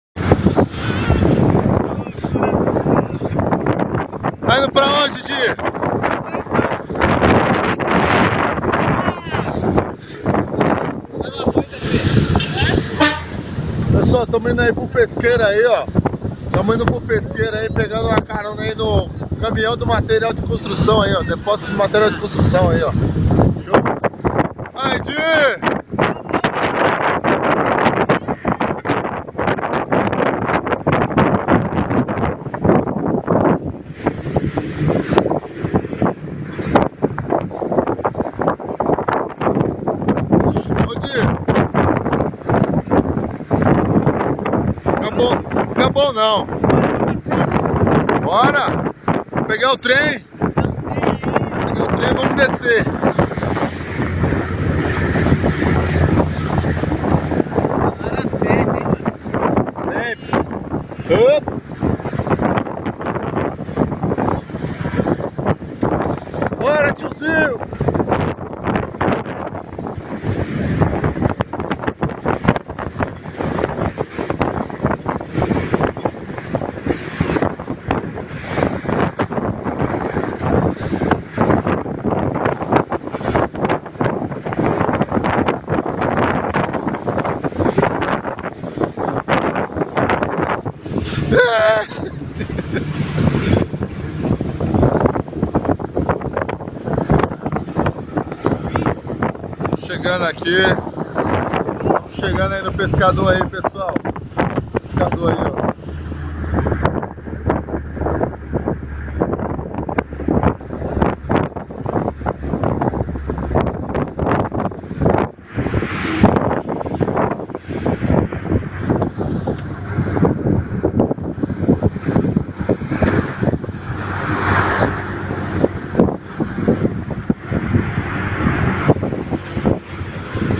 Pegando carona de caminhão , rolezinho no barragem
Cada dia é uma nova história e um novo rolê , obrigado Senhor